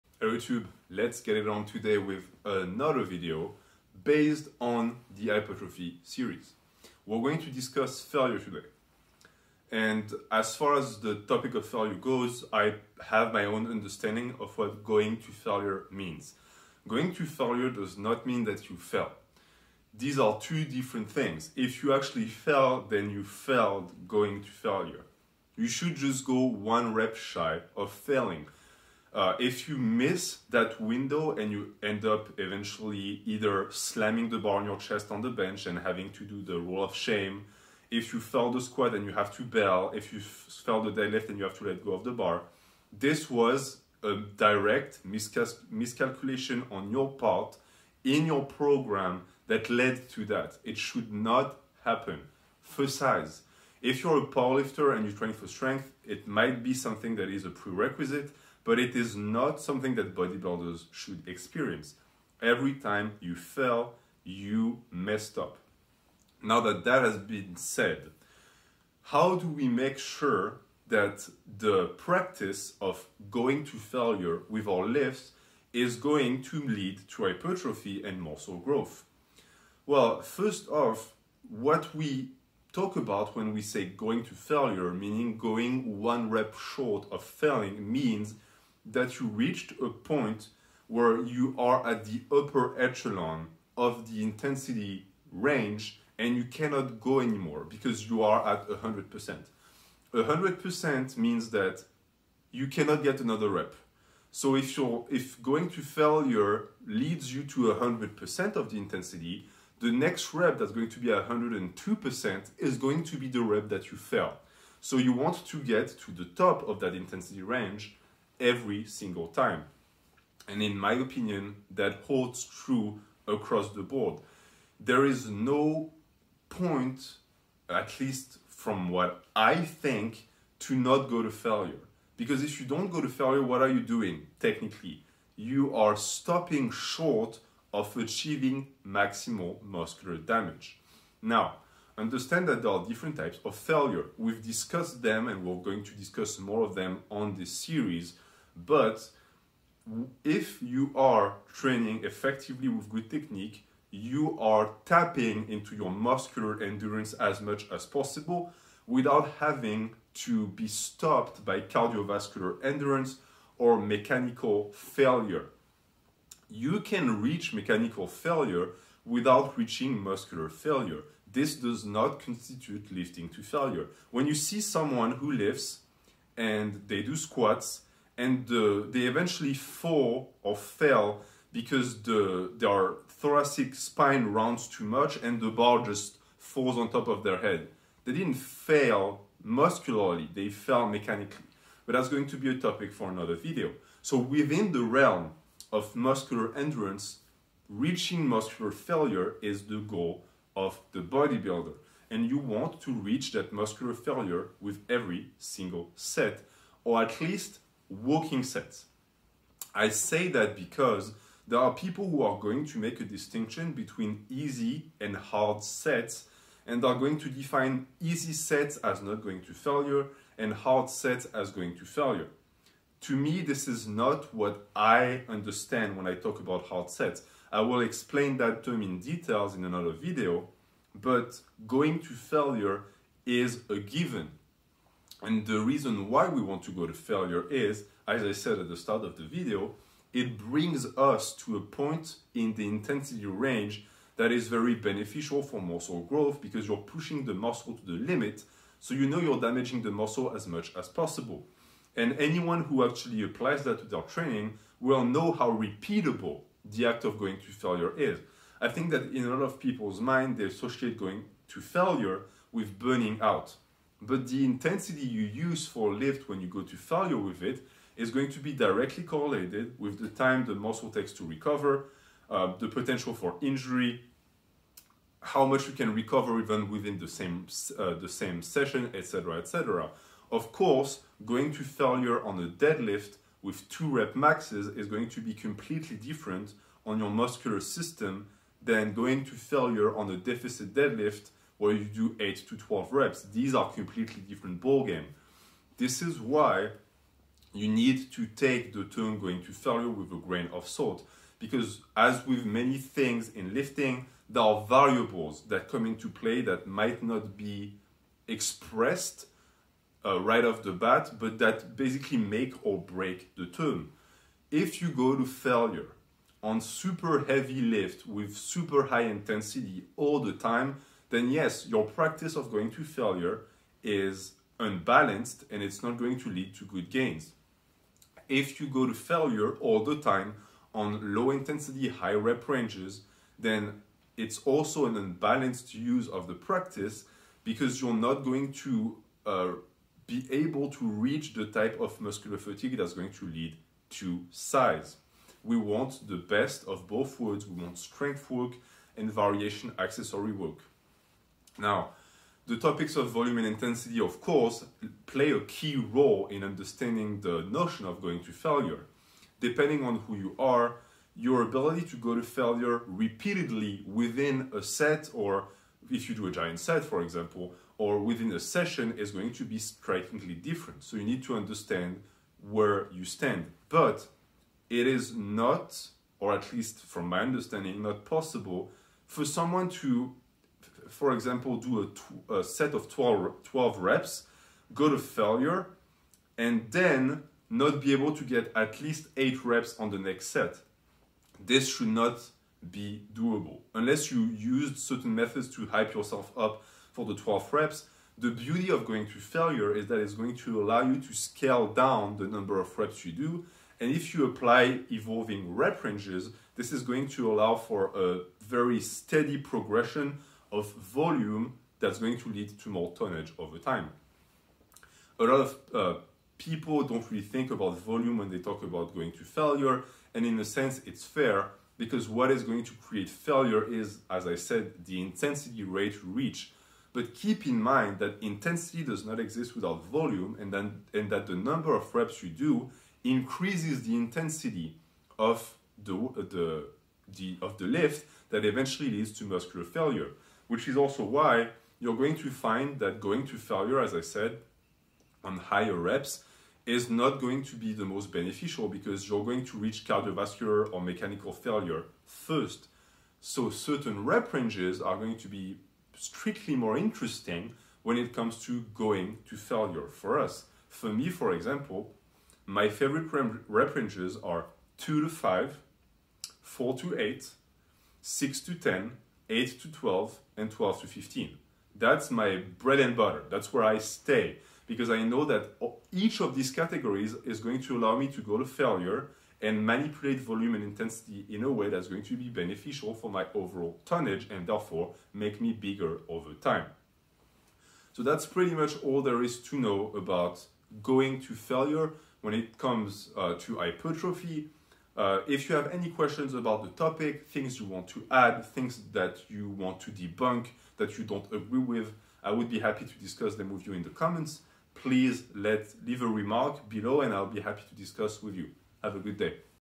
The hypertrophy series: the importance of going to failure for muscle building
Going to failure is critical to build muscle, and here's why. On the same topic will also be discussed the implications this method creates in terms of frequency and overall tonnage control to keep recovery in check. The concept of baseline will also be a big one